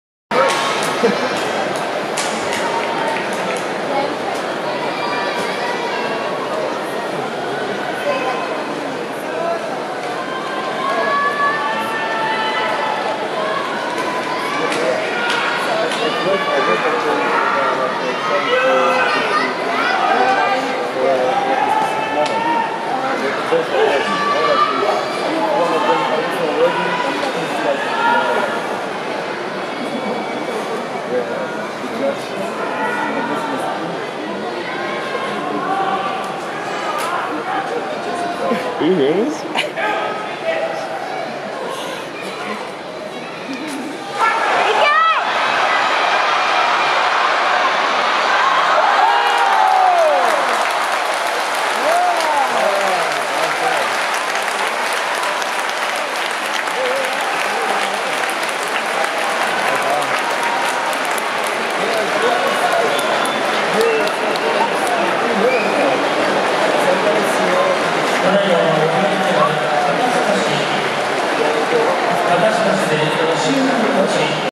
Ishiura (6-6) - Ikioi (8-4) , Sumo : Natsubasho '17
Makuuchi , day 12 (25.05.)
(l) Ishiura - M 11w, Miyaginobeya
(r) Ikioi - M 6w, Isenoumibeya
Kimarite : Katasukashi